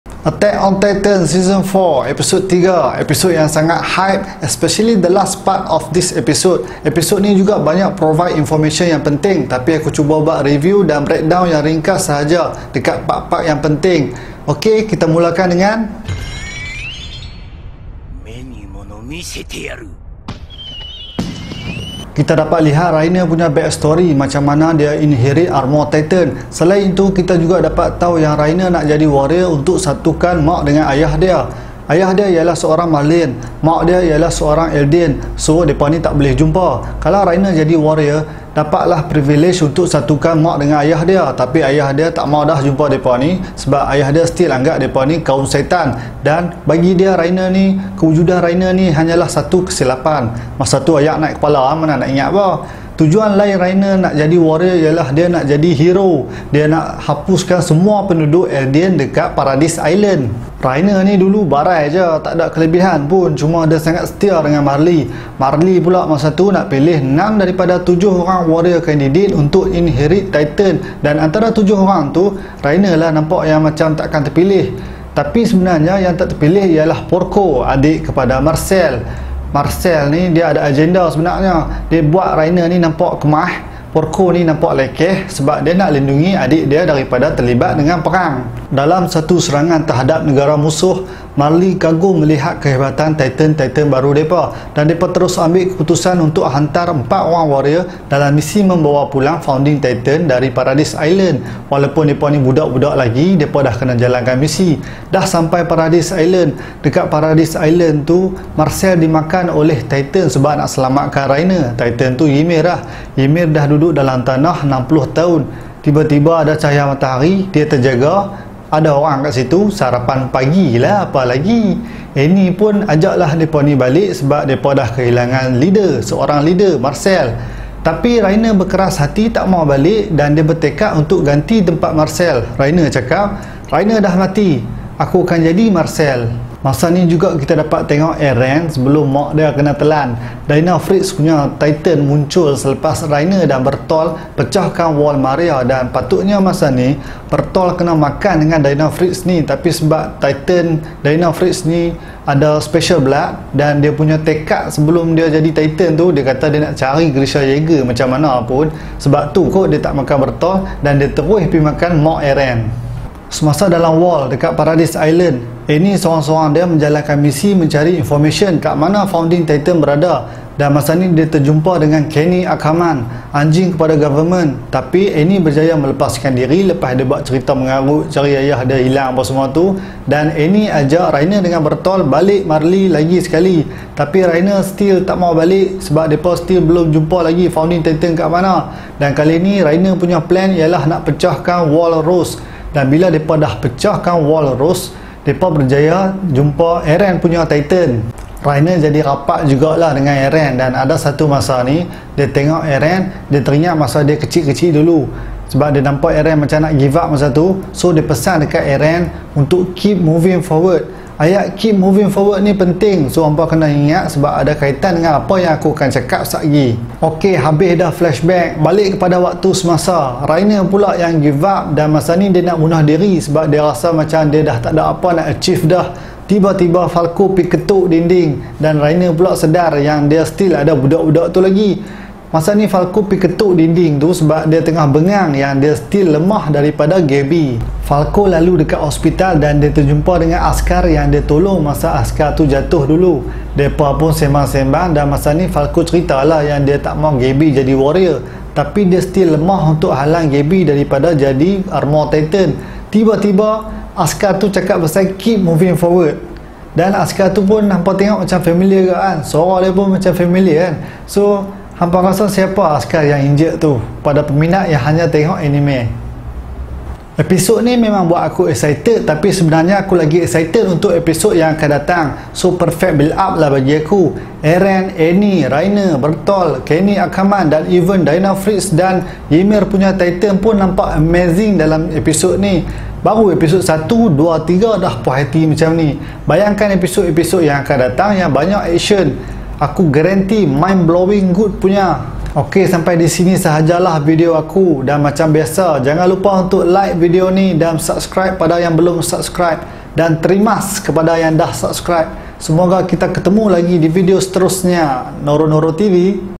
Review dan Penjelasan Anime - Attack on Titan Episode 3 Final Season
Video ini merupakan review dan penjelasan anime Attack on Titan Episode 3 Final Season daripada Noro Noro TV. Kami mengulas semula isi kandungan episode ini dan memberikan penjelasan yang berkaitan dengan episode ini. Dalam episode 3 kita dapat lihat flashback kisah Reiner dan kawan-kawan Eldian dari Marley yang lain. Bagaimana mereka inherit titans dan pergi ke Paradis Island. Di saat akhir, seseorang telah buat comeback. Siapakah dia? Jumpa lagi di video akan datang!